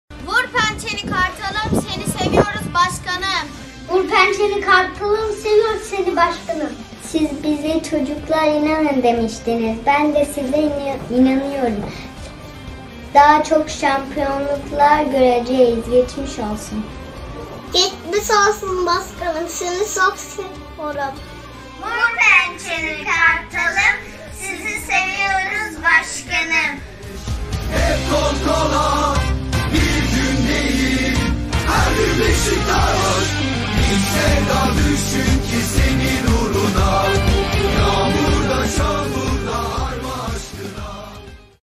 Vur Pençeni Kartalım, Seni Seviyoruz Başkanım
BJK Kabataş Vakfı Öğrencilerinden Beşiktaş Jimnastik Kulübü'ne Geçmiş Olsun Mesajı